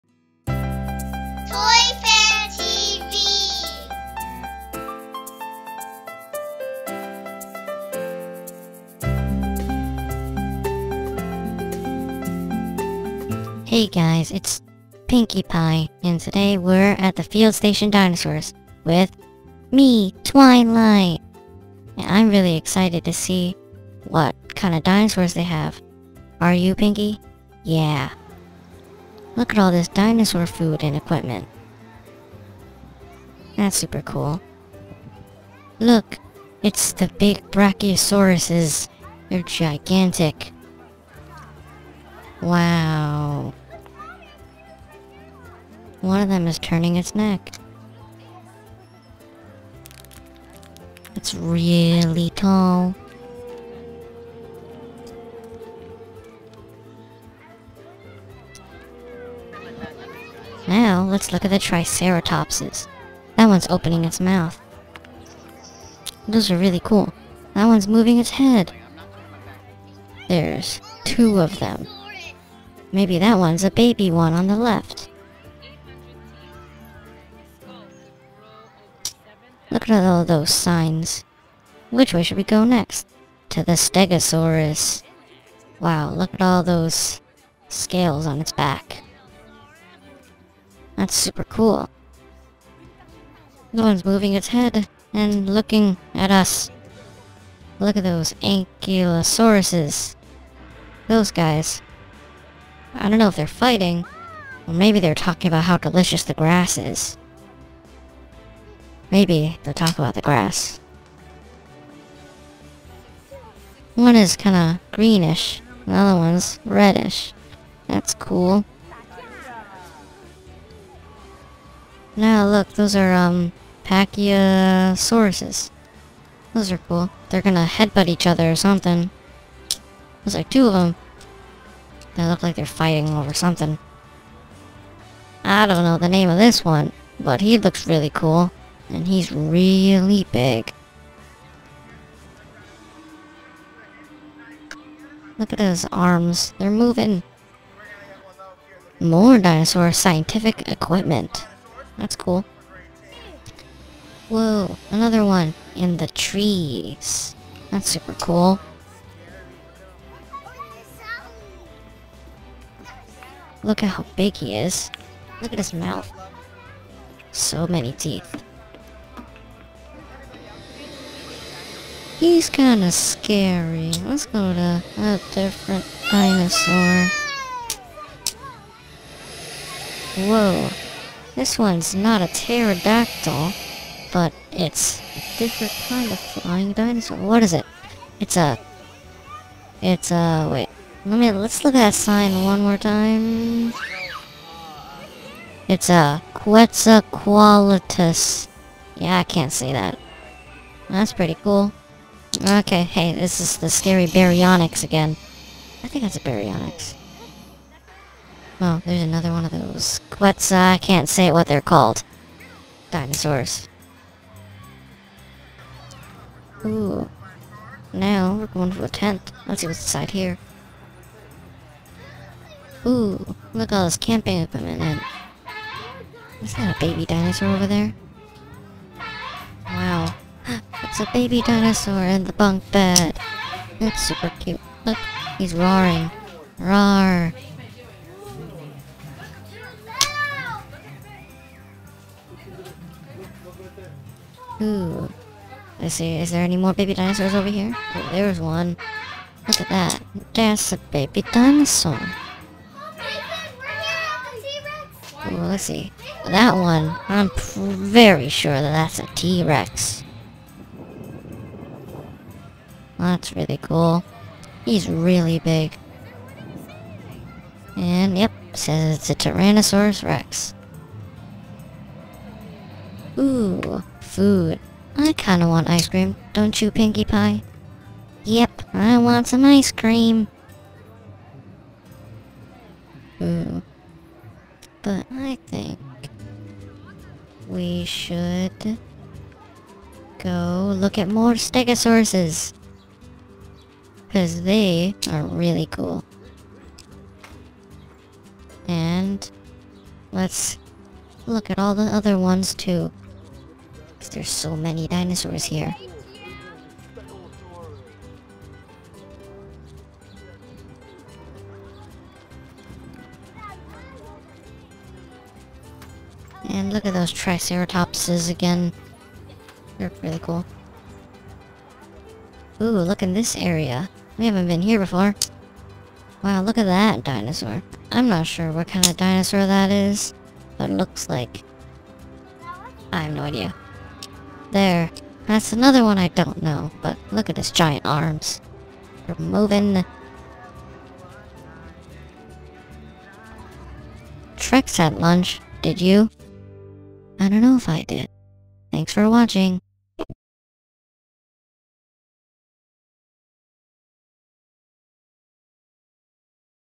Field Station Dinosaurs Leonia New Jersey, with Twilight and Pinkie
Twilight and Pinkie went to Field Station Dinosaurs in Leonia, New Jersey. Follow along and see all the huge dinosaurs. T-Rex, triceratops, brachiosaurus, baby dinosaurs and more!
Summer 2017